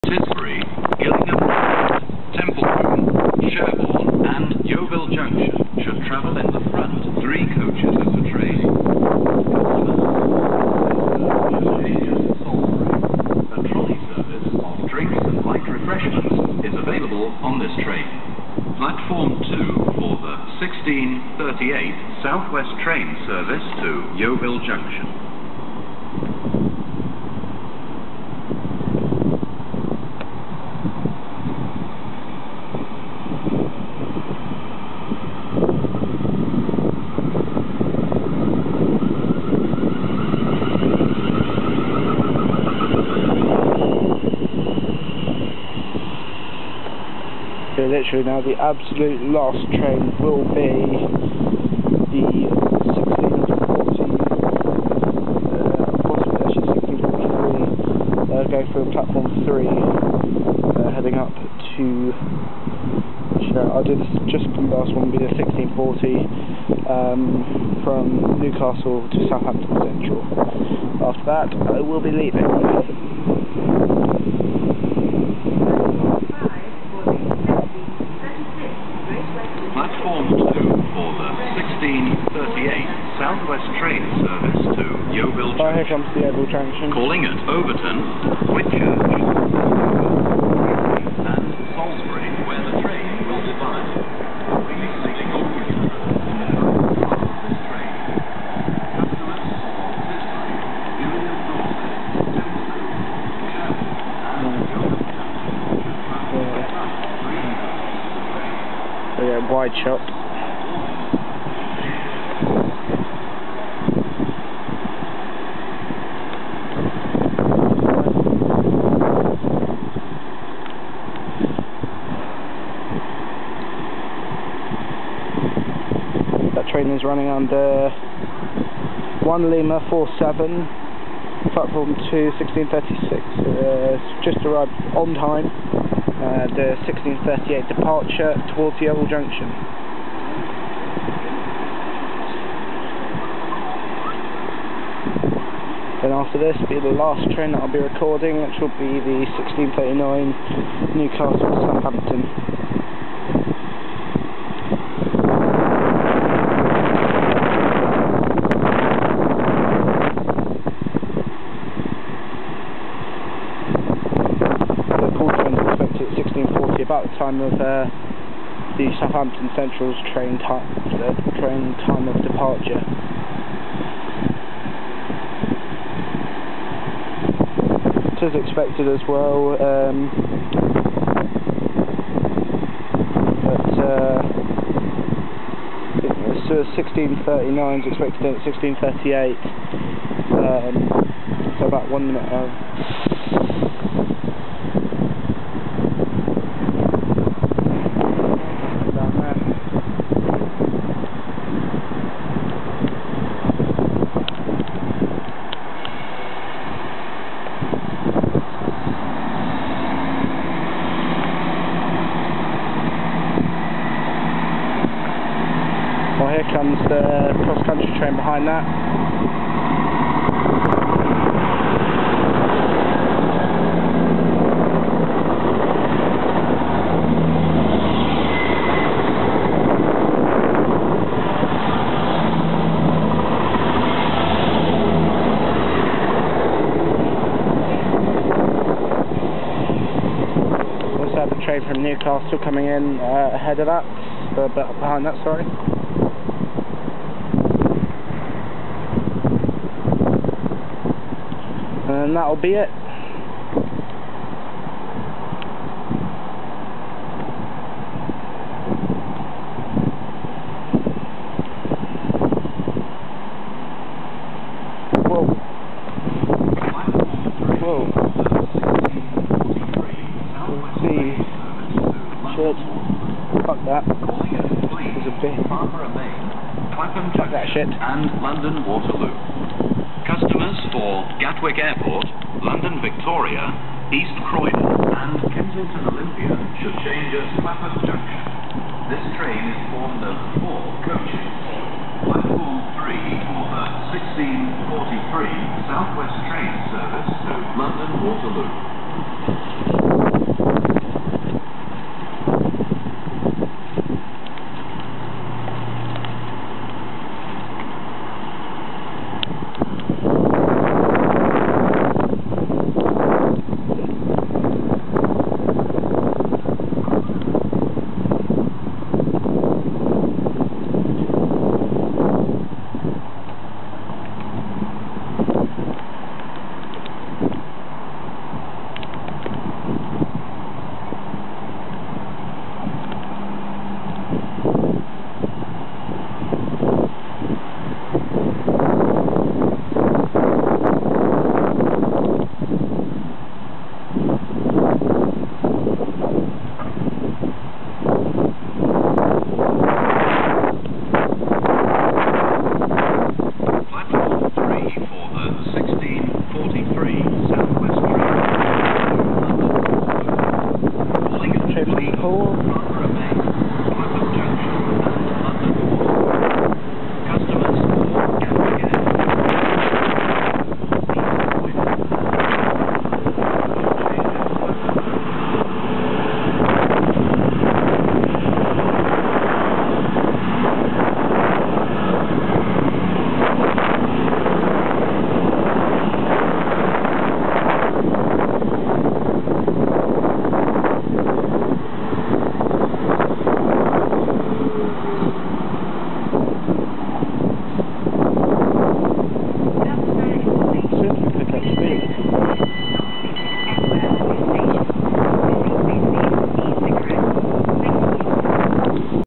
LOADS OF ACTION @BASINGSTOKE - 08/07/16
Class 444 Departs P3
Class 159 Arrives and Departs P2
Class 220 Arrives and Departs P1
Class 450 Arrives at P3